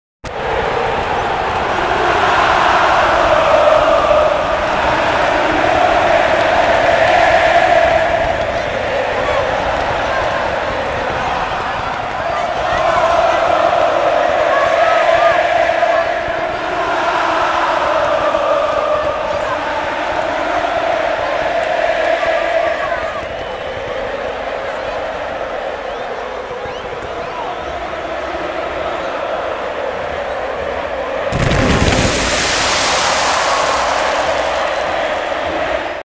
Impresionante recibimiento de la hinchada de Talleres frente a River
Recibimiento de la hinchada del equipo cordobés frente a River en una nueva fecha de la superliga argentina